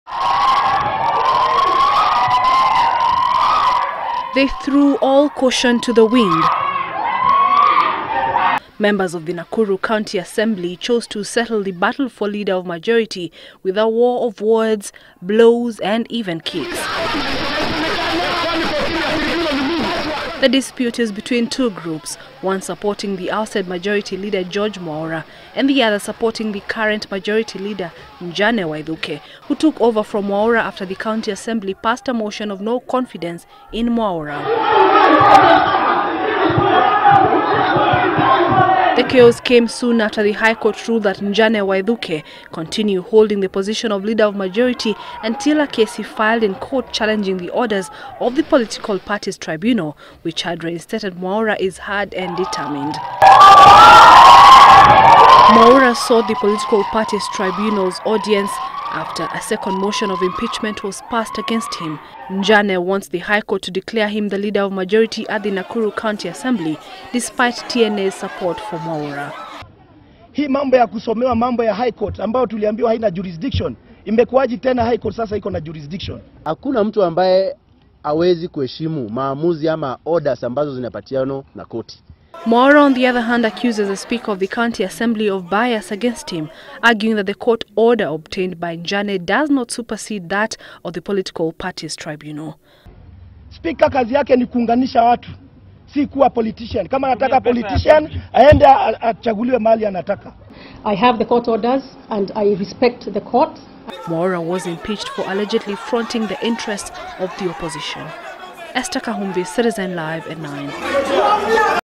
Nakuru MCAs Trade Blows over Majority Leader’s Post
Activities at the Nakuru County Assembly were violently disrupted after chaos erupted following a clash between two groups battling for the position of the leader of majority in the assembly. Trouble began after the speaker of the county assembly declared Njane Wathuki as the leader of the majority. Njane obtained a court order after he moved to court challenging the reinstatement of George Mwaura who had been impeached by the county assembly.